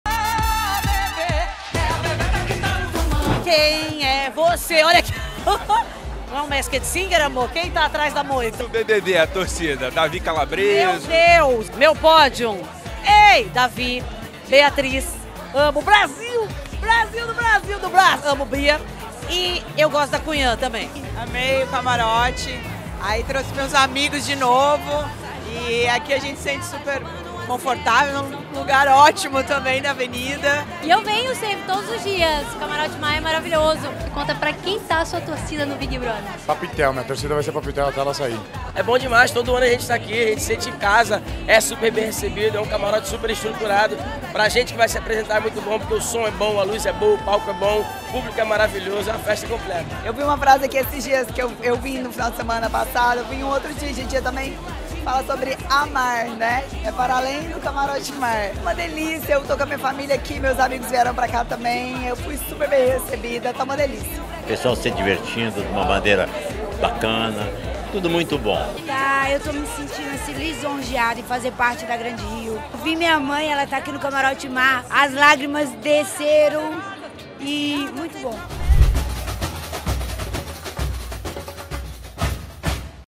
Famosos caem no samba na Sapucaí
Confira quem fez a festa no primeiro dia de desfiles do Grupo Especial no Camarote Mar.